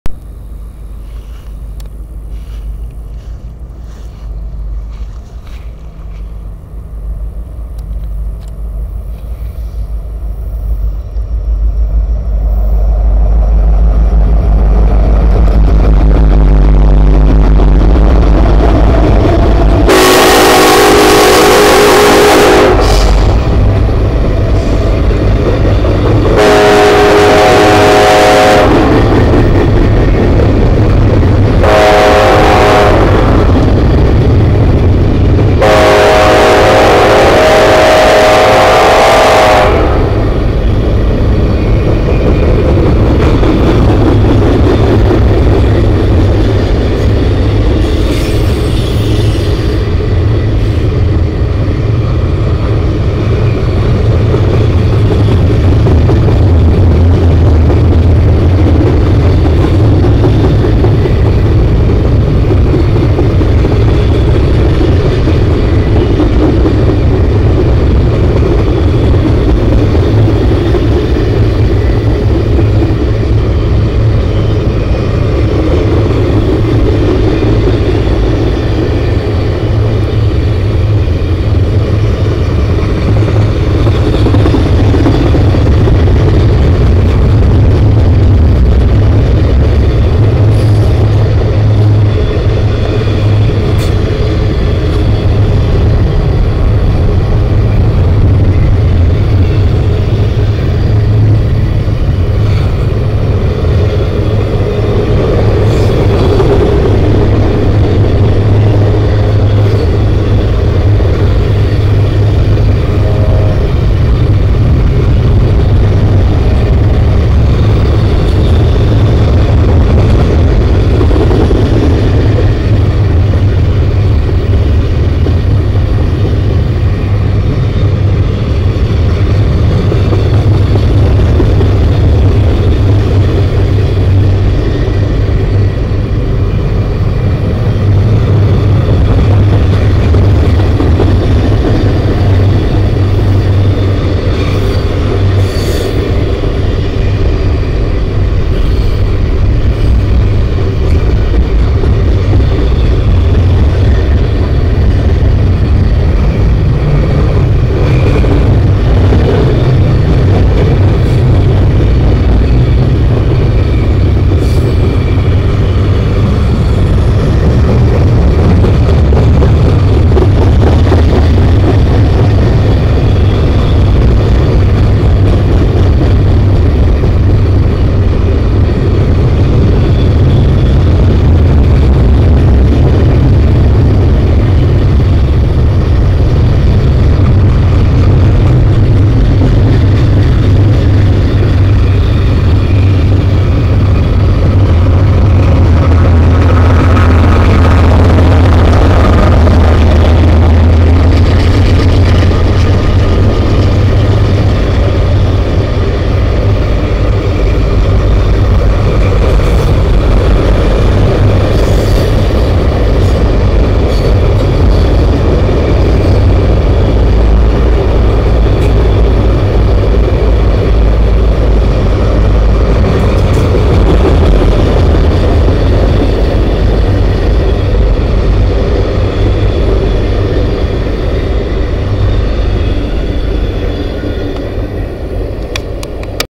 NS#278
NS #278  as they roll east bound on the a_line near mp88.2